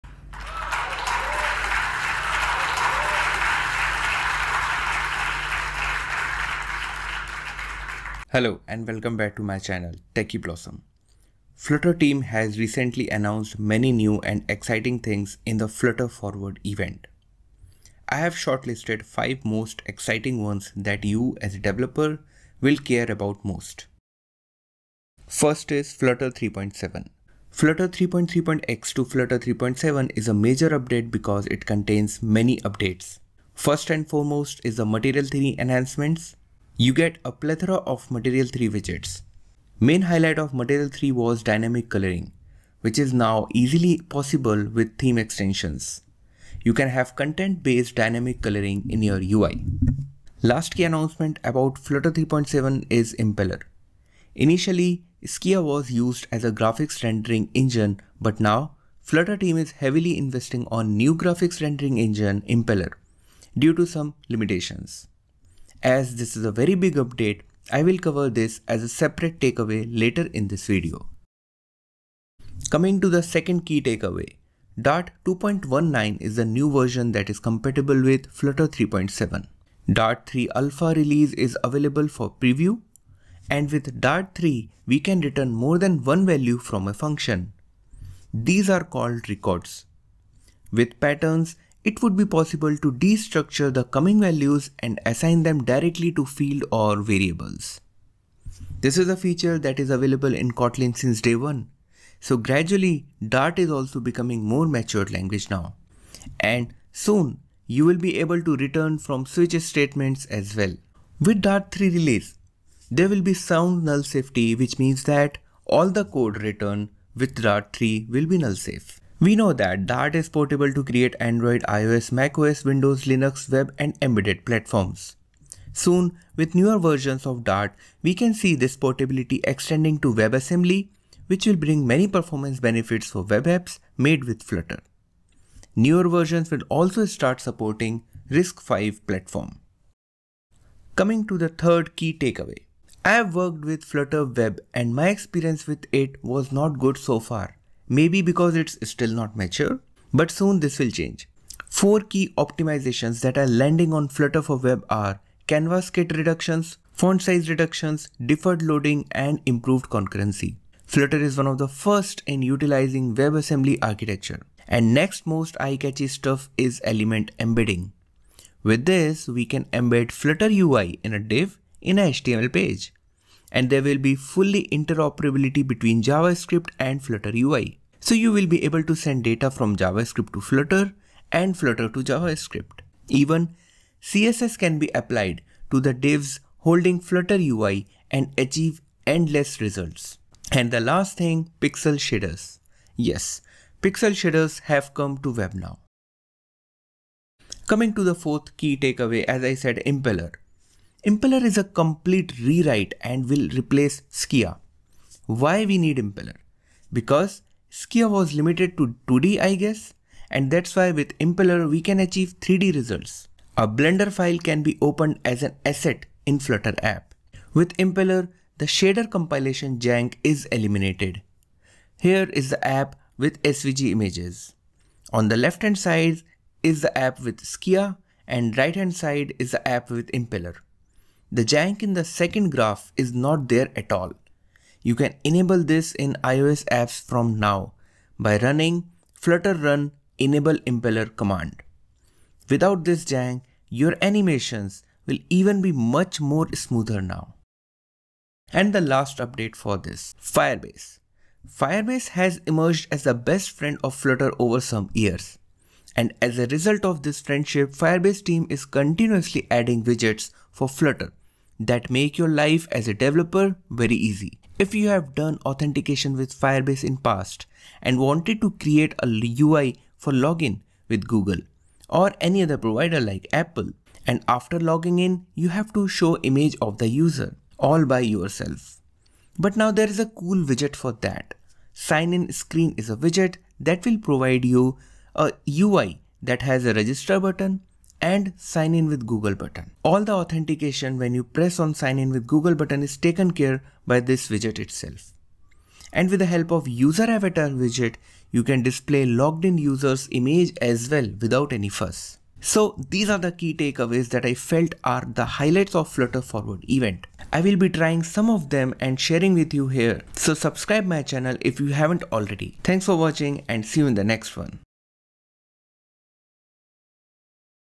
5 KEY TAKEAWAYS From FLUTTER FORWARD | WHY switch to FLUTTER?
In this video, I will be sharing 5 key takeaways from Flutter Forward event that happened on 25th January 2023.